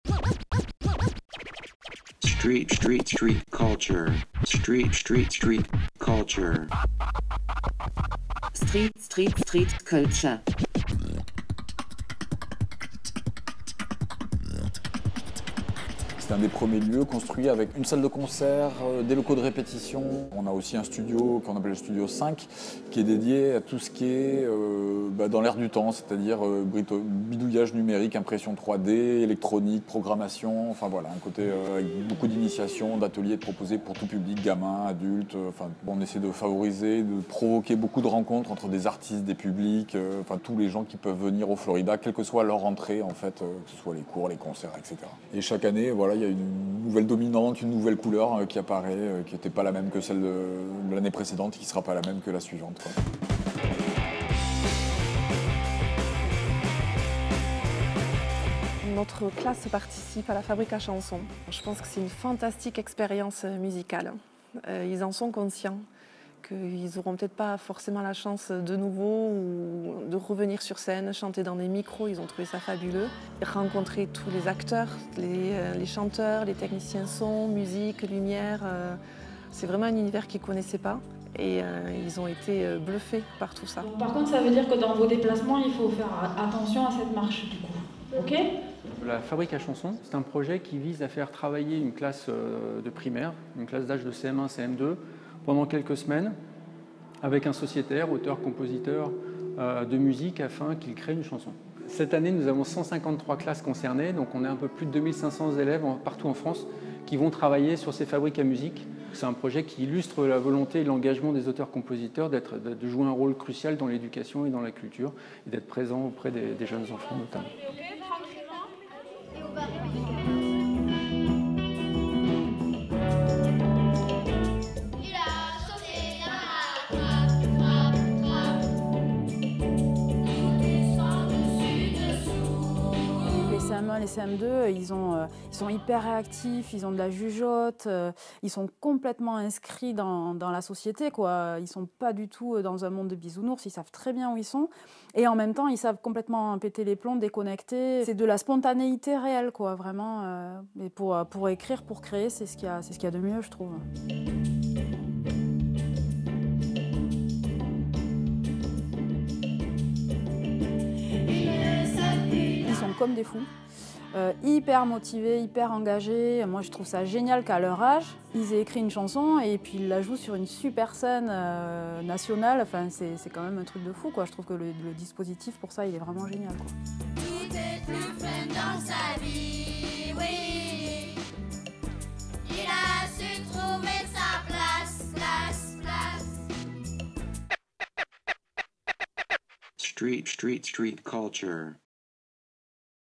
Street Culture #30 : LE FLORIDA (Agen - 47)
Le Florida, mythique salle agenaise ! Ce temple de la musique comble tous les publics. De nombreux événements sont organisés tout au long de l’année. Activités pour les musiciens en herbe, ateliers d’éveil, fabrique à chansons, rencontres entre artistes et public…Le Florida, lieu emblématique pour la culture.